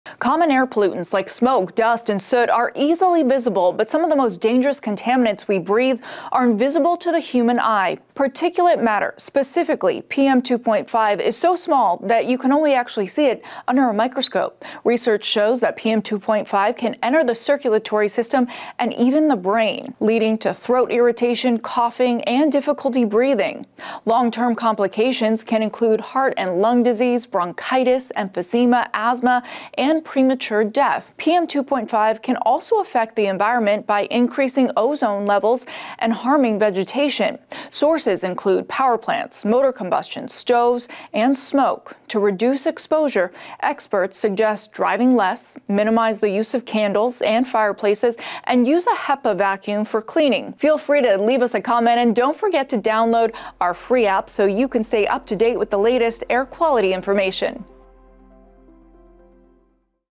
Health impacts of PM2.5
Learn what PM2.5 is and how it affects your health.